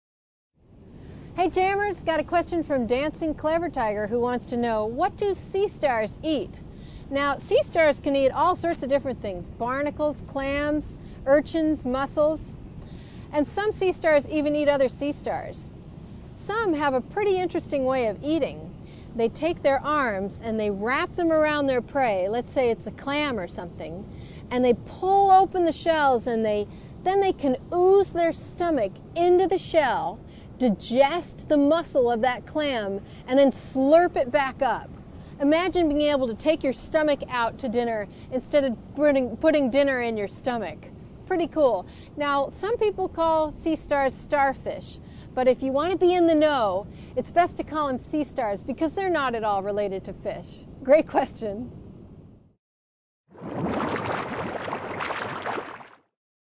Animal Jam - Ask Tierney: What do sea stars eat?
Dr. Tierney Thys reveals interesting facts of how the sea stars eat and what their diets consist of.
Find more videos like this one and so much more